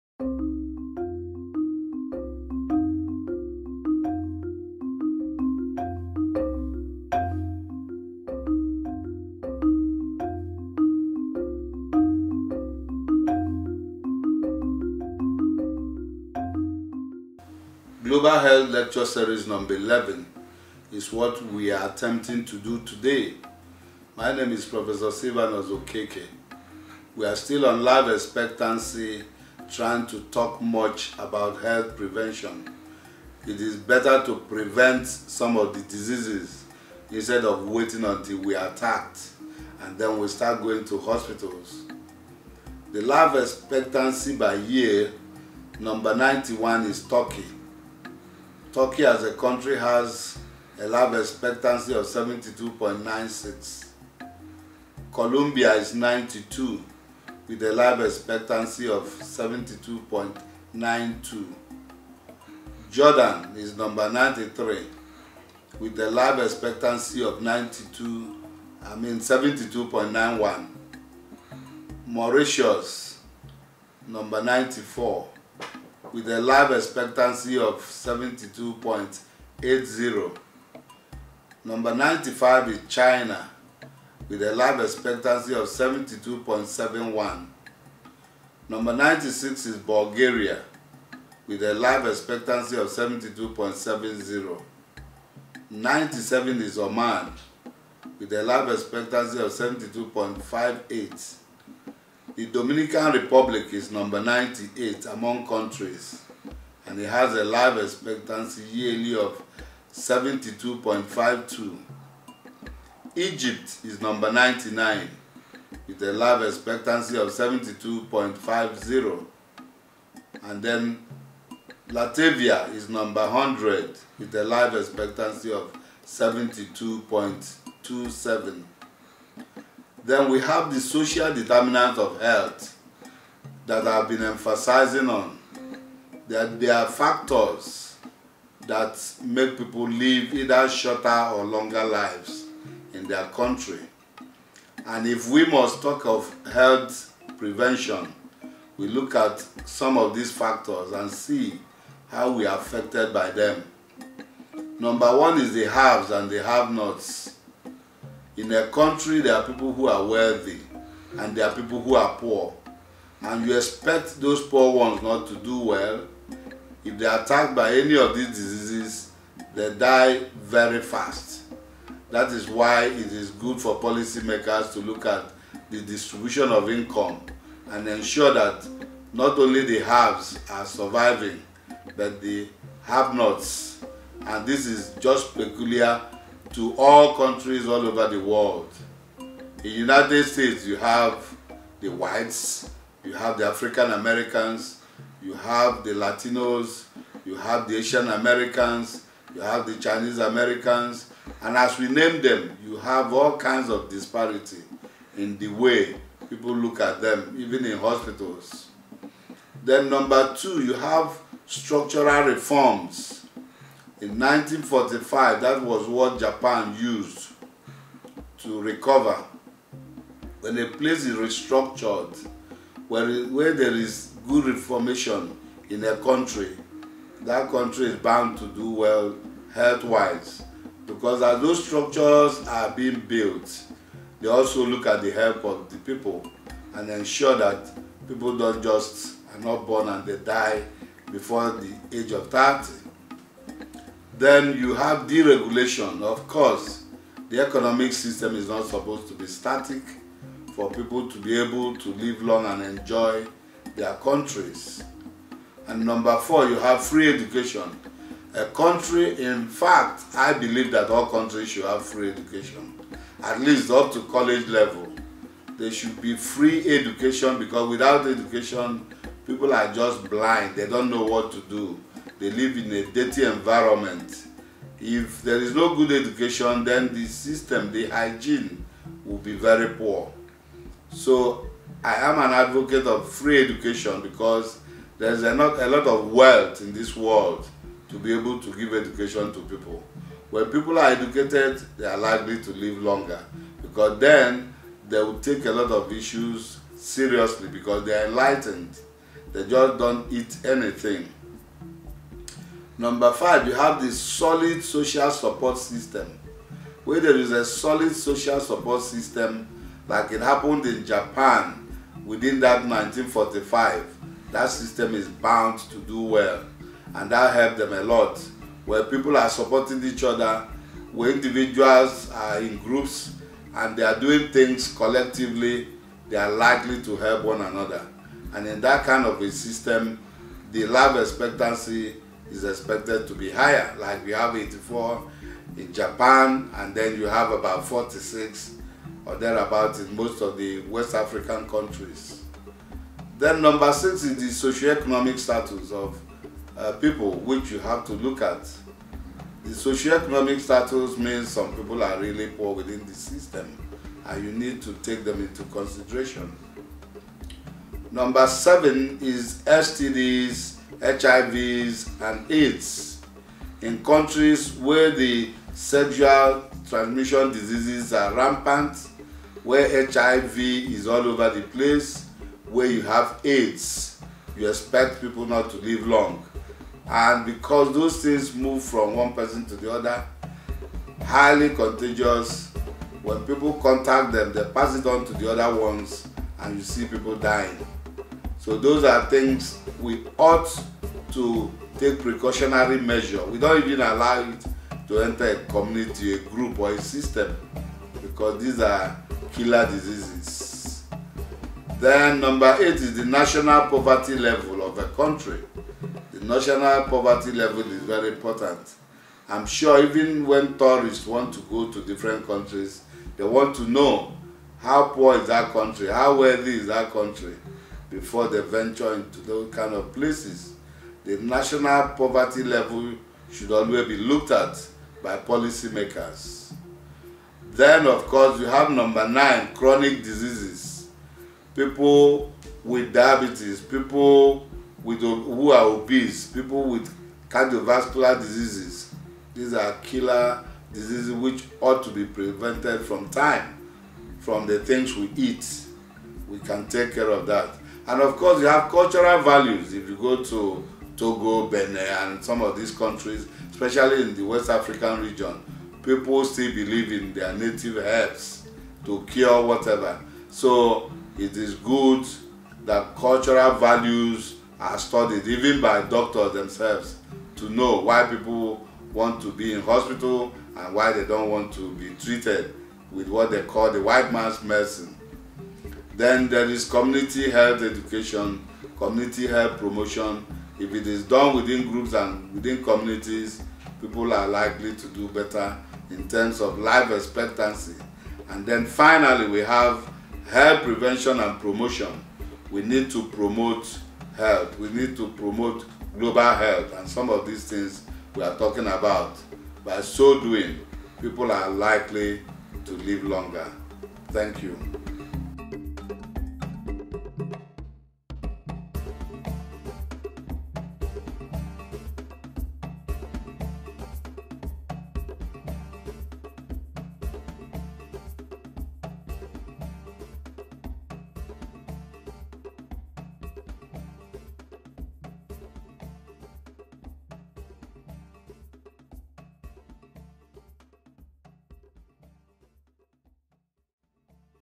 Prof. Silvanus Okeke - Global Health Lecture Series # 11 " I AM AN ADVOCATE OF FREE EDUCATION"
There is enough wealth globally to have free education. Good education will ensure good cleanliness of our society, good governance, professional health inspectors, good public health officers, an so on.